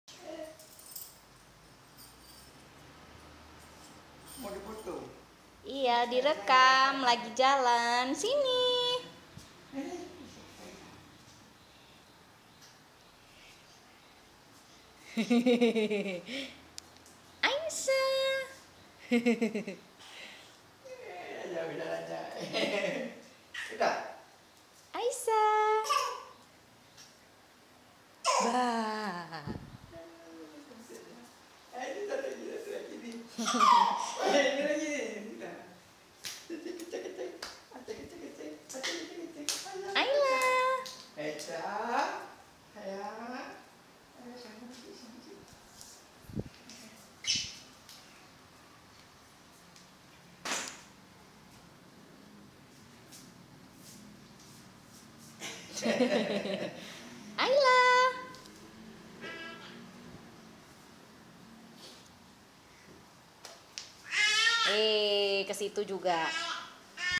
10 month old Twins Baby learn to walk (bayi kembar 10 bulan belajar jalan)
10 month old Twins Baby learn to walk (bayi kembar 10 bulan belajar jalan)